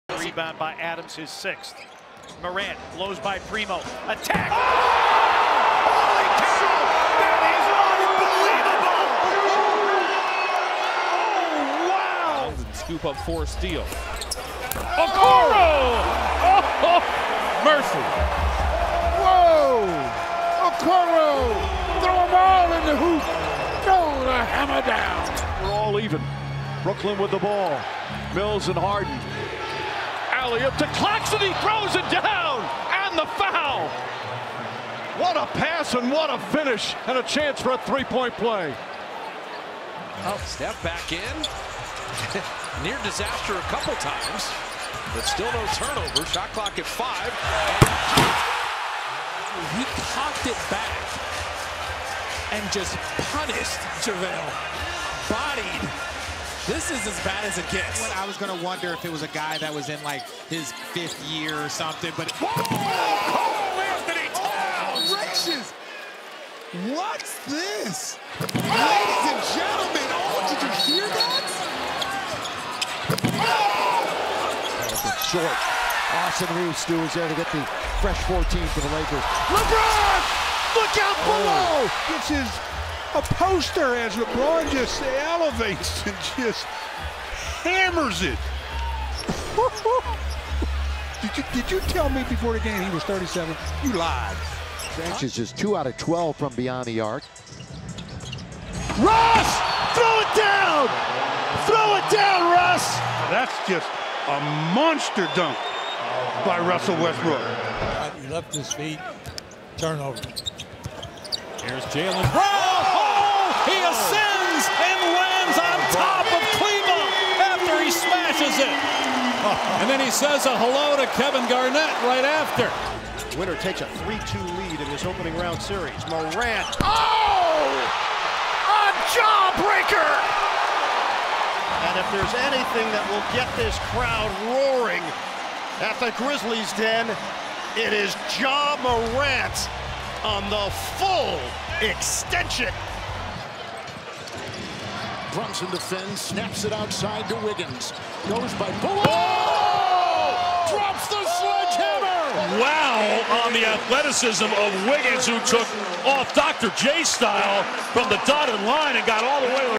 2021-2022 NBA Sezonu'nun En İyi 10 Smacı! Enfes Smaçlar!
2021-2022 NBA Sezonu boyunca yapılan en iyi 10 smaç! Enfes smaçları kaçırmayın!